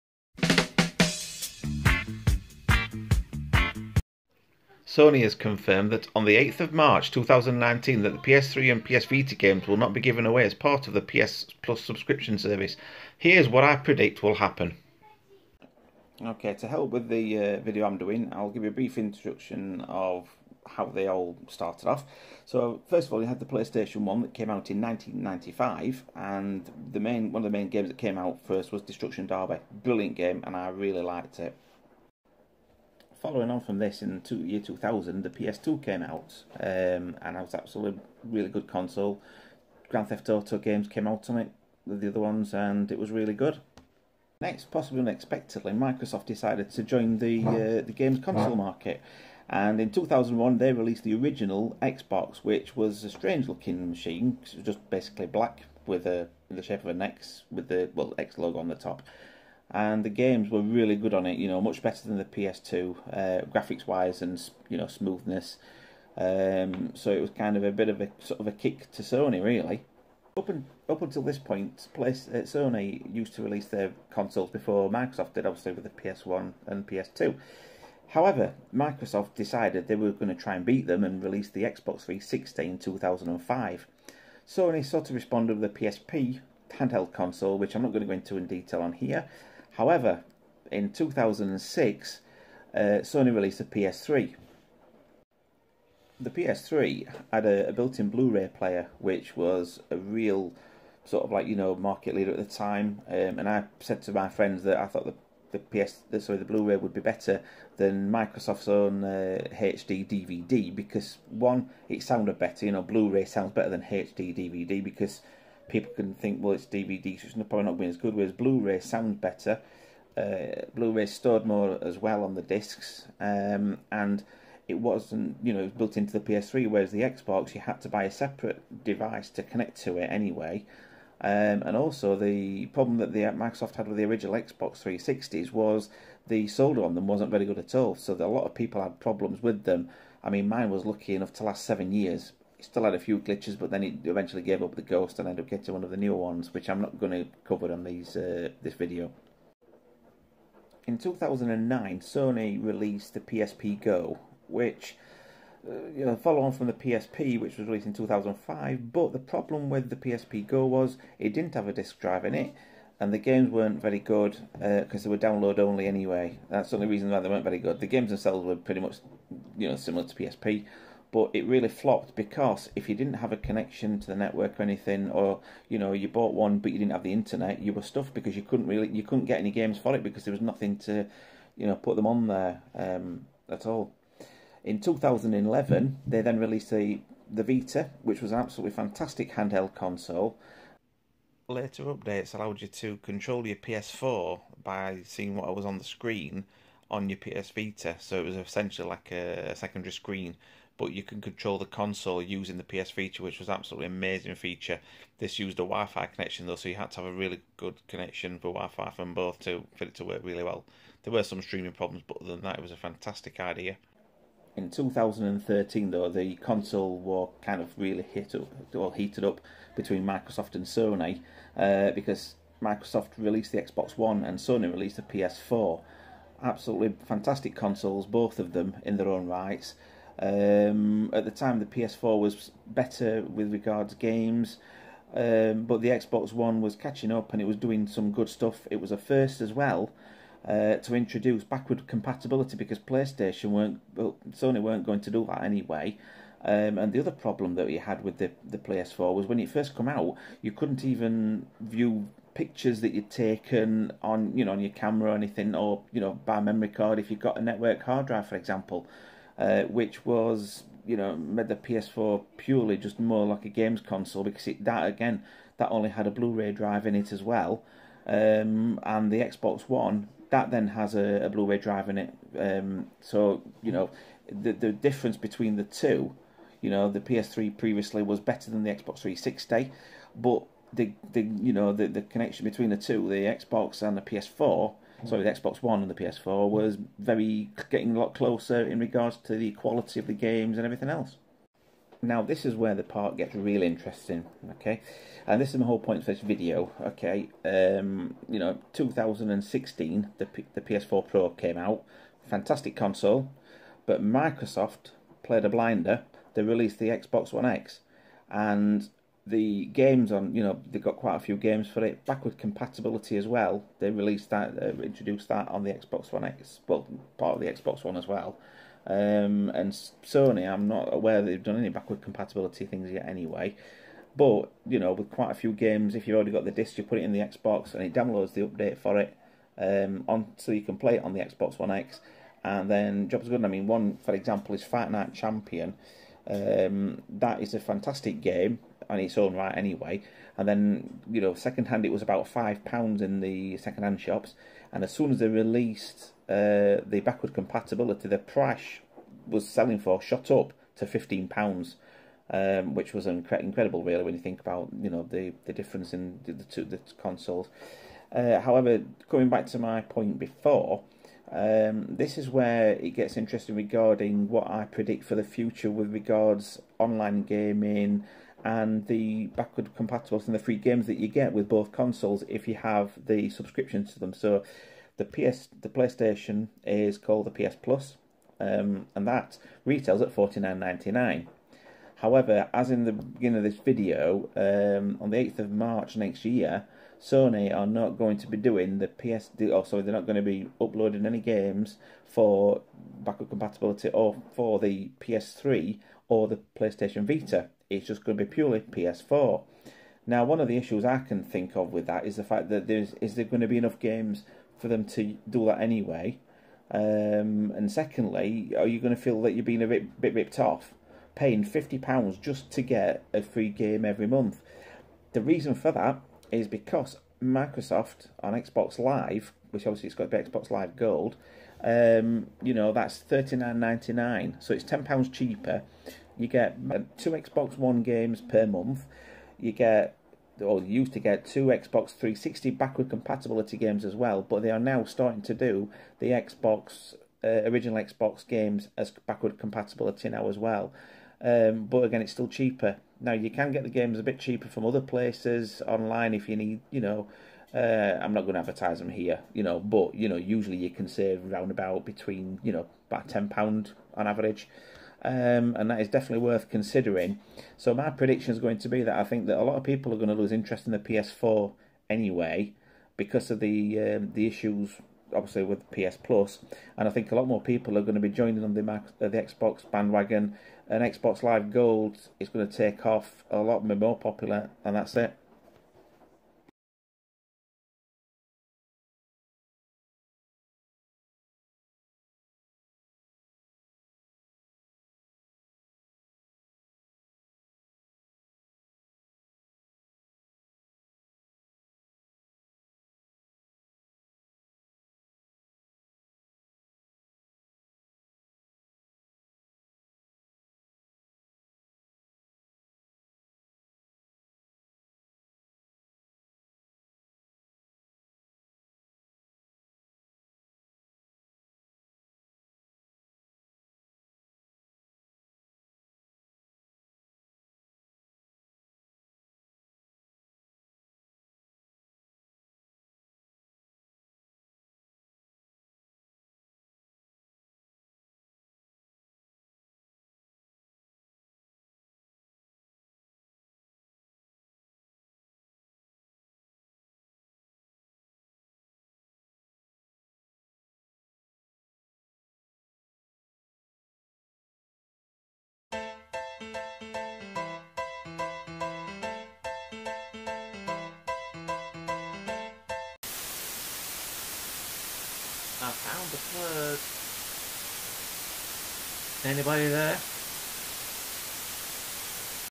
Sony are going to stop Vita and PS3 games in March 2019 in the monthly subscription
On 8th of march Sony are going to be removing the PS3 and the Vita games to download so there will only be 2 x PS4 games that will be available to download monthly. Whether Sony will reduce the price or not will be decided by them at a later date but will they would do I think? Watch my video and my prediction of what will happen after March 2019.